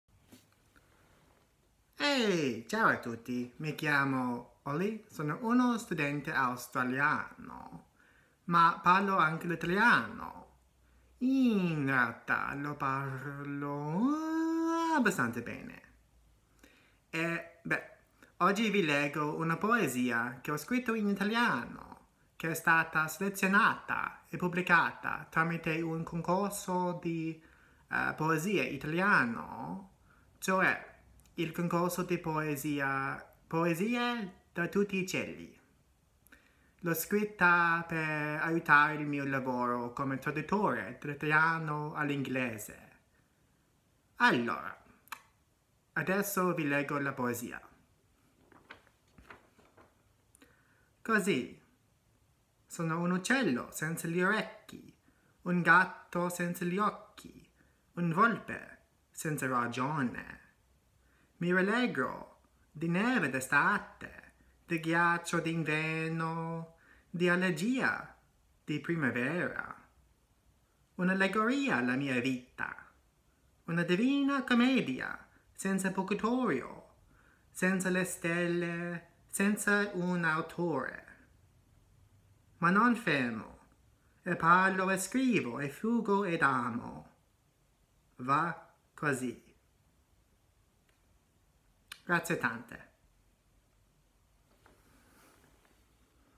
Madrelingua inglese leggendo una poesia scritta in italiano
La mia poesia ‘Cosi’, selezionata per il concorso di poesia ‘Poesie Da Tutti i Cieli’